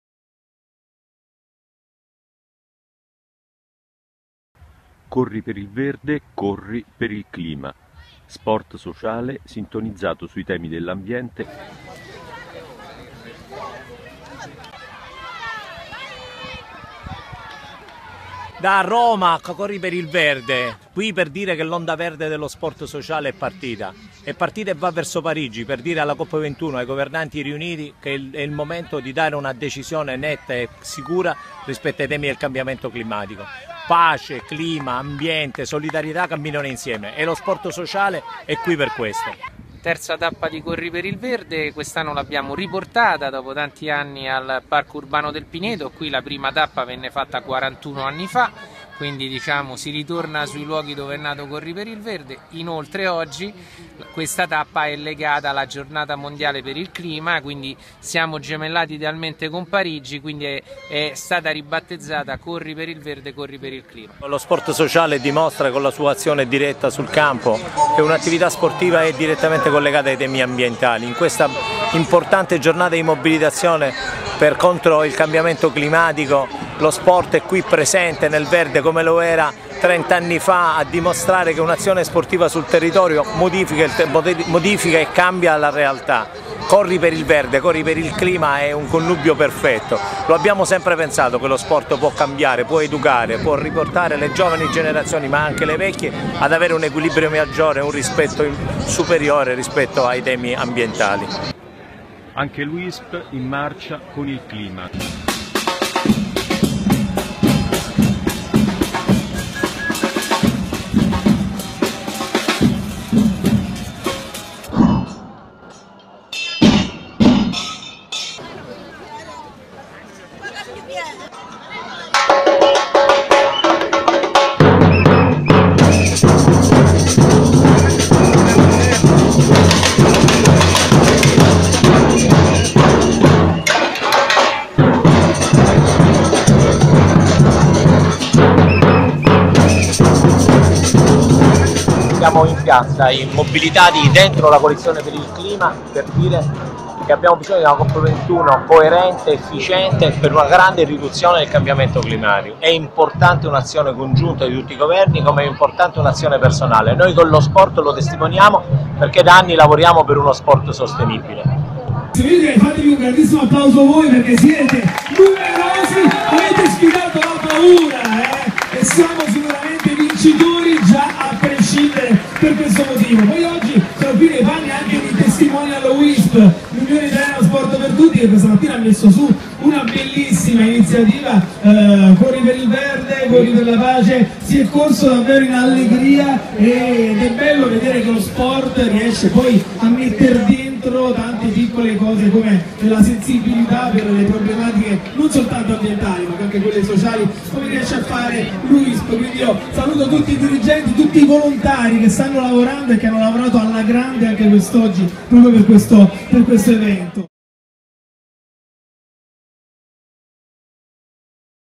L'Uisp e lo sport sociale in Marcia per il clima e per la pace
Domenica 29 novembre a Roma l'Uisp protagonista in mattinata con "Corri per il verde" e nel pomeriggio ai Fori Imperiali con giochi in piazza per bambini e in Marcia per il clima. Intervengono Santino Cannavò, responsabile politiche ambientali Uisp, Gianluca Di Girolami, presidente Uisp Roma e Luca Abete, inviato di Striscia l a Notizia, sul palco al termine della Marcia per il clima.